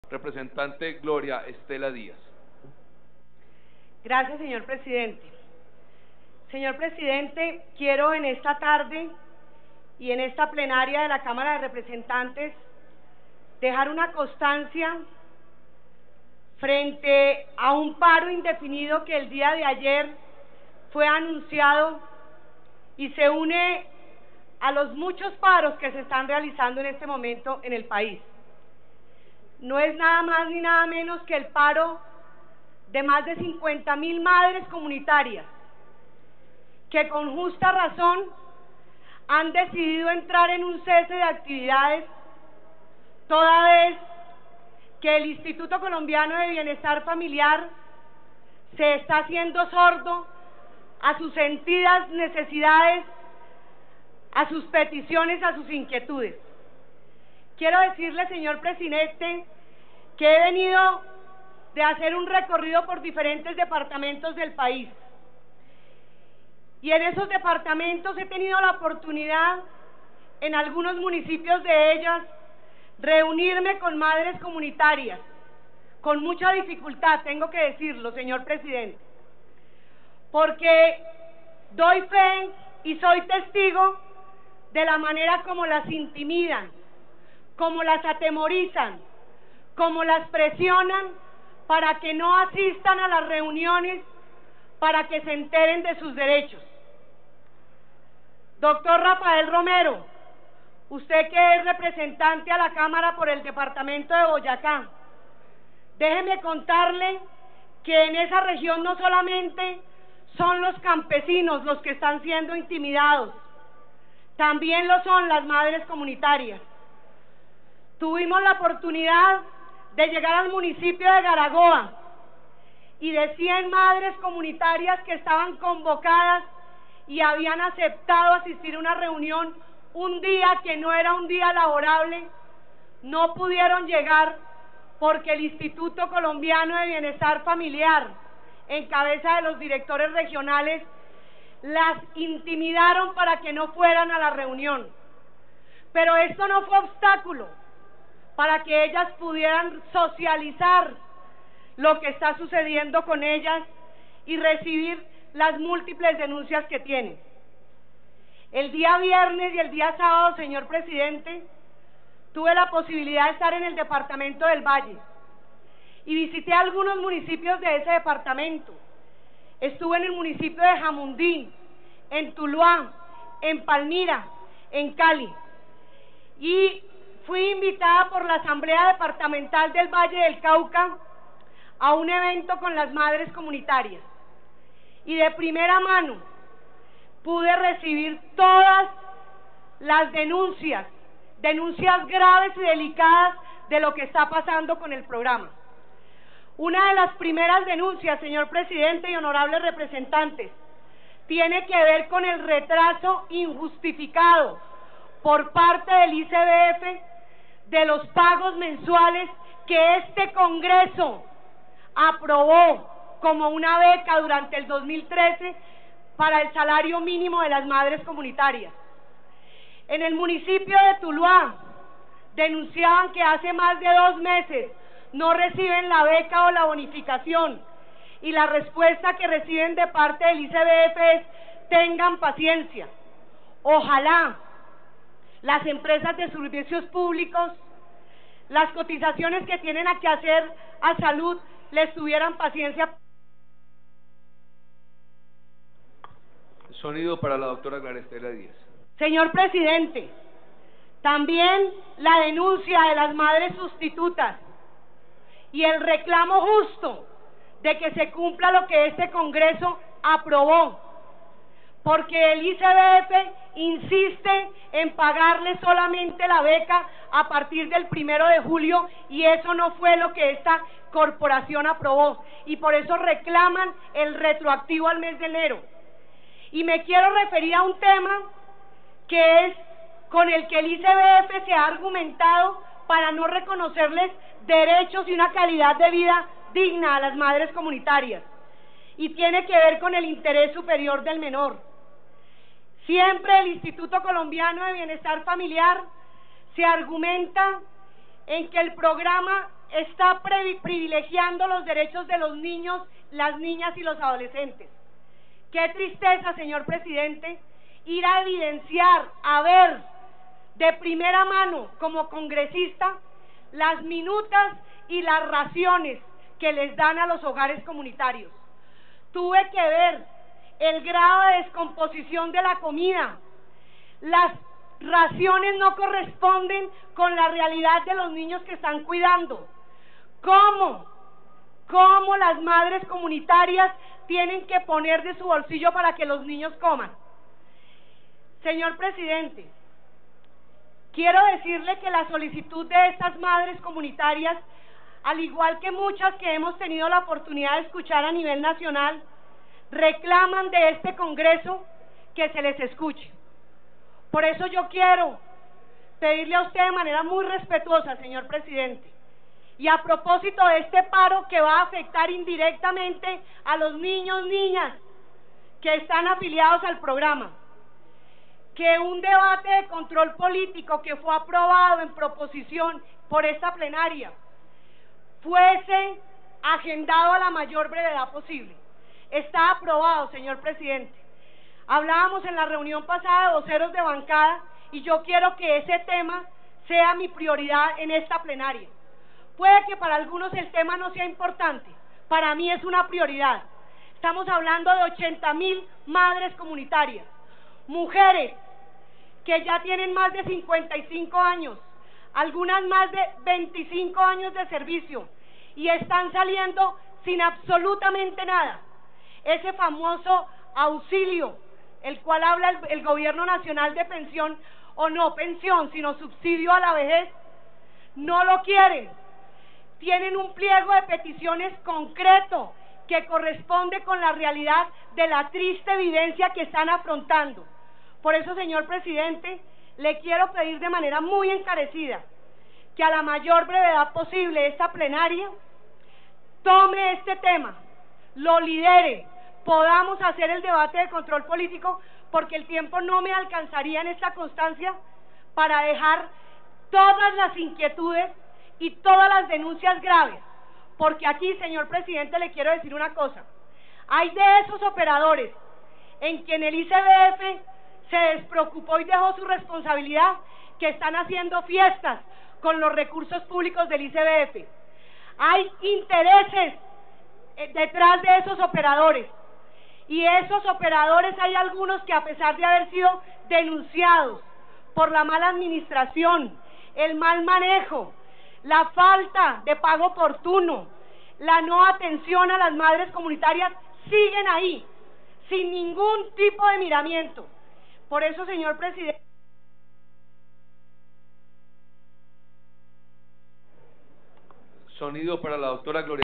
Constancia Frente al Paro Indefinido de las Madres Comunitarias - Plenaria de Cámara
El pronunciamiento de la Representanta a la Cámara por Bogotá, Gloria Stella Díaz (MIRA)  en la Plenaria de la Cámara sobre el paro indefinido y justificado de las Madres Comunitariasl del País. Agosto 27 de 2013